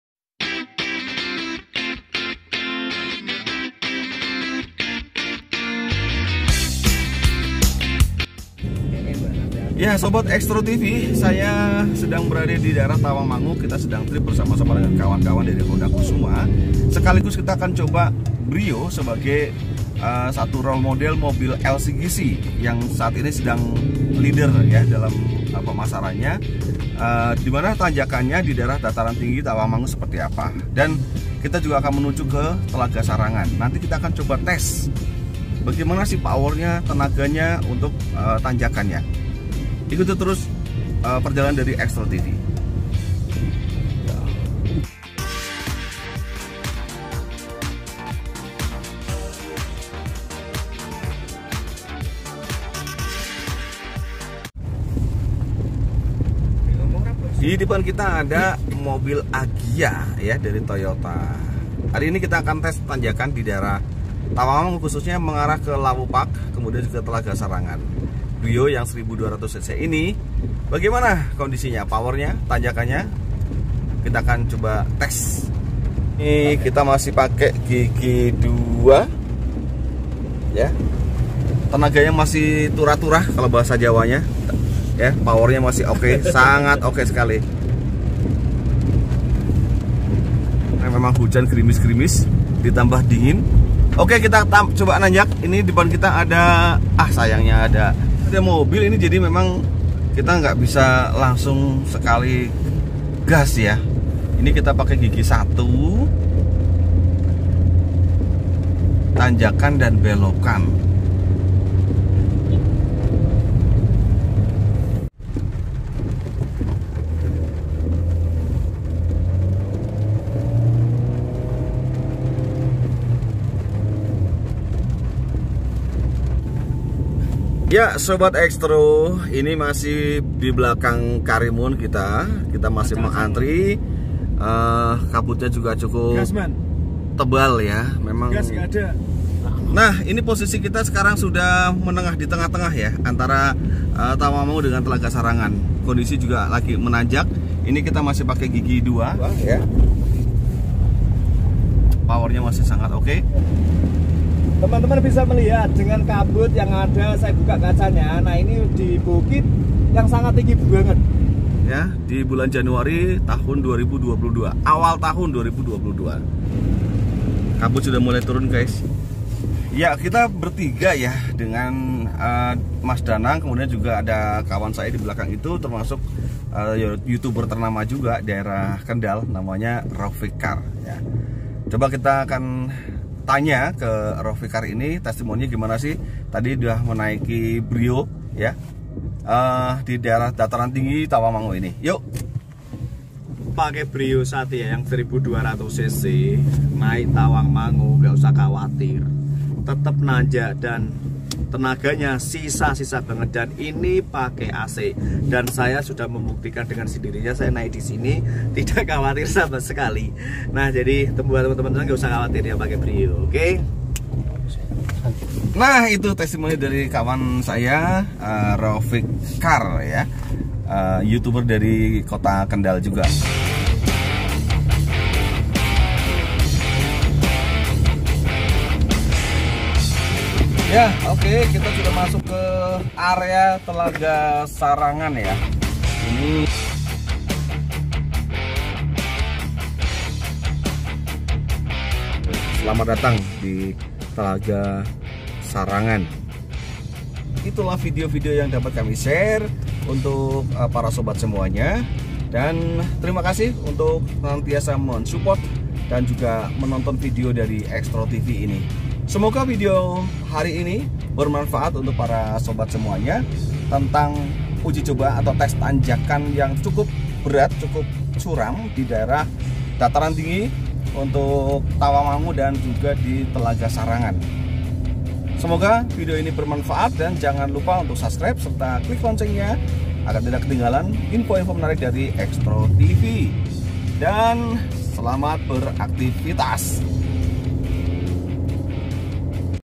Pakai BRIO Libas Tanjakan Curam ❗Tawangmangu & Telaga Sarangan ❗AC Nyala
Beberapa hari lalu ,kami membuktikan bahwa Brio Manual sangat power full di Tanjakan Dataran Tinggi , Tawangmangu dan Telaga Sarangan

info test Drive dan Pembelian
hubungi

Maryono
Honda Kusuma

Kunjungi Video kami yang lain
Terimakasih atas supportnya ...

test drive my new car brio in the night,